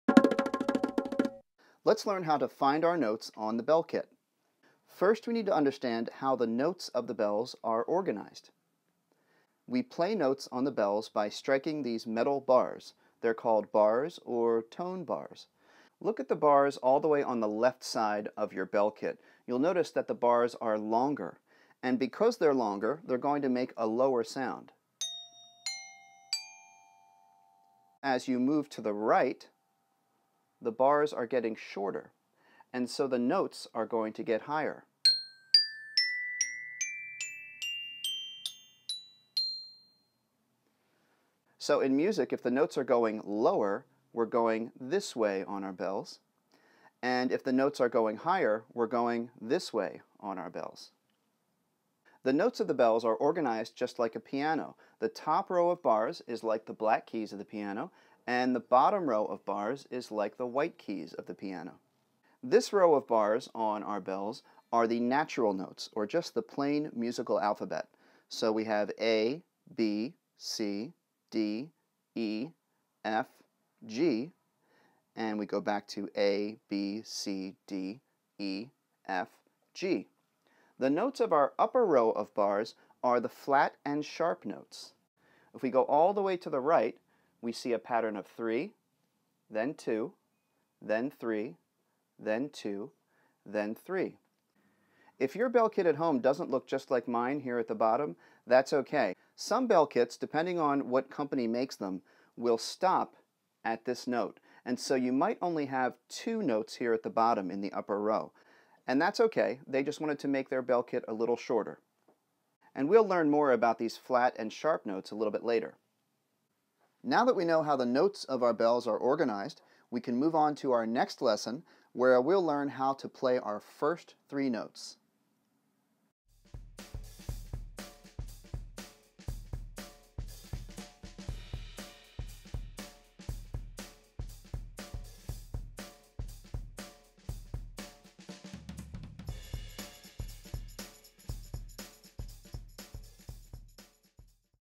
Bells Beginner Lesson - Finding Your Way Around the Keyboard
In this beginner bells lesson, we learn how the notes of the bells are organized.